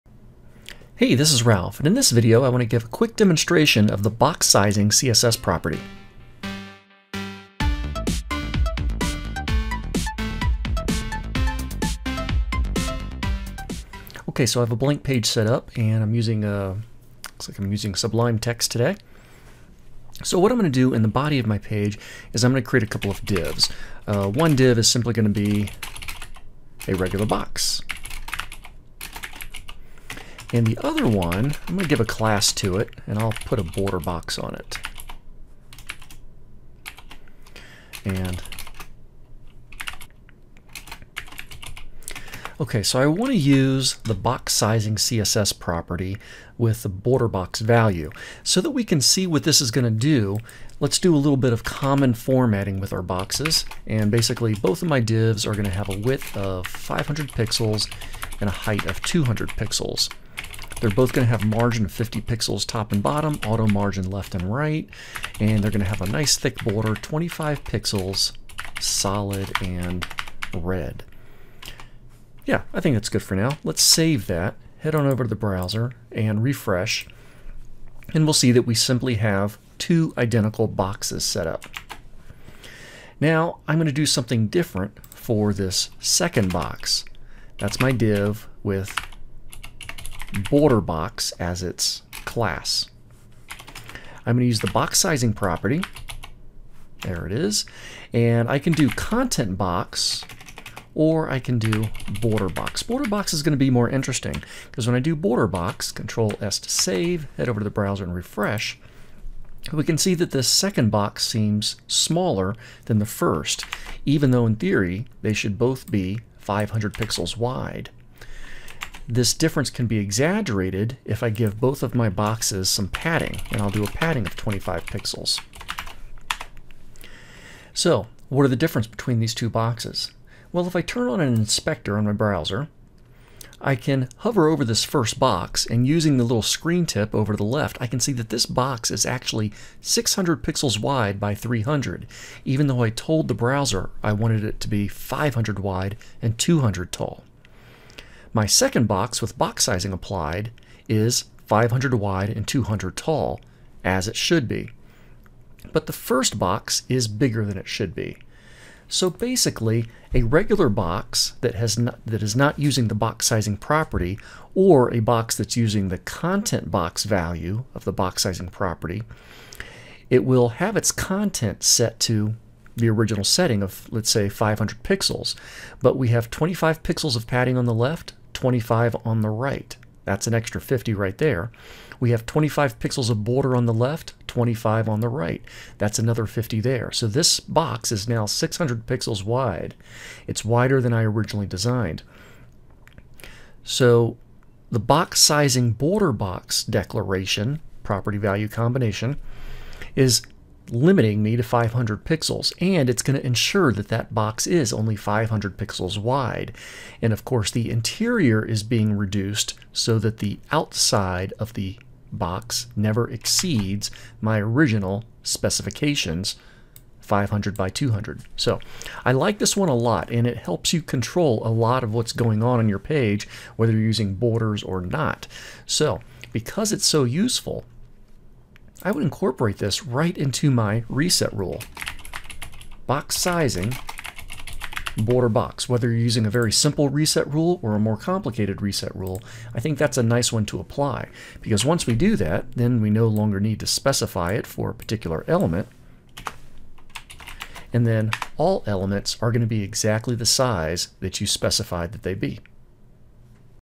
Control Block Size with the CSS Box-sizing Property
Use box-sizing: border-box to help ensure that a block's width is set regardless of padding or border sizes.